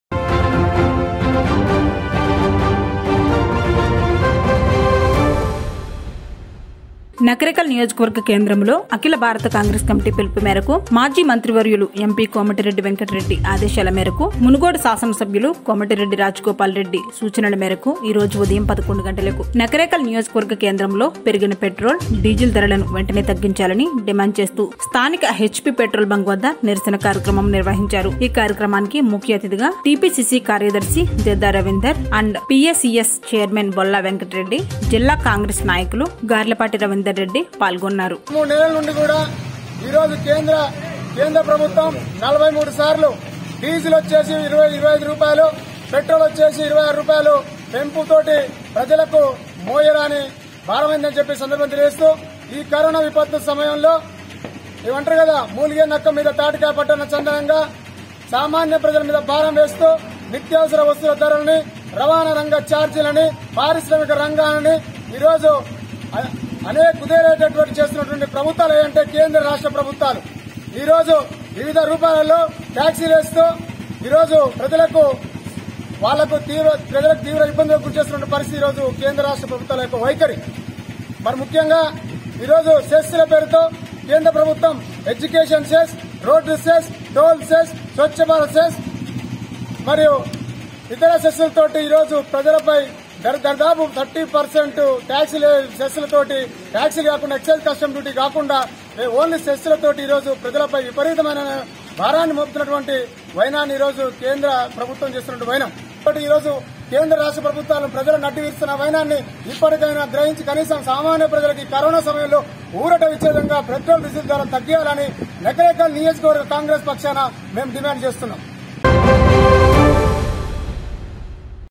నల్గొండ జిల్లా:నకిరేకల్ అఖిల భారత కాంగ్రెస్ కమిటీ పిలుపు మేరకు పెరిగిన పెట్రోల్,డిజిల్ ధరలను వెంటనే
నల్గొండ జిల్లా:నకిరేకల్ అఖిల భారత కాంగ్రెస్ కమిటీ పిలుపు మేరకు పెరిగిన పెట్రోల్,డిజిల్ ధరలను వెంటనే తగ్గించాలని hp పెట్రోల్ బంక్ వద్ద నిరసన కార్యక్రమం.